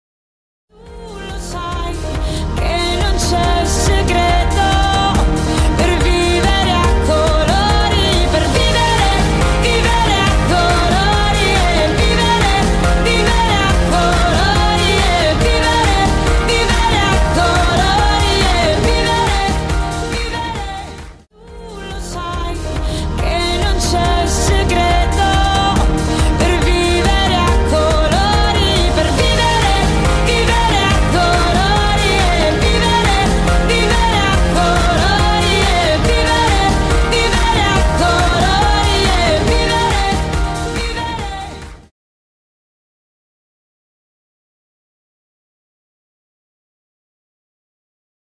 Baila Dance scuola di ballo danza arti marziali Roma
Video promo della scuola di ballo e danza Baila Dance, centro polifunzionale a Roma zona Cornelia Battistini sita in Via Gregorio XI 211, direzione artistica Claudio Massaccesi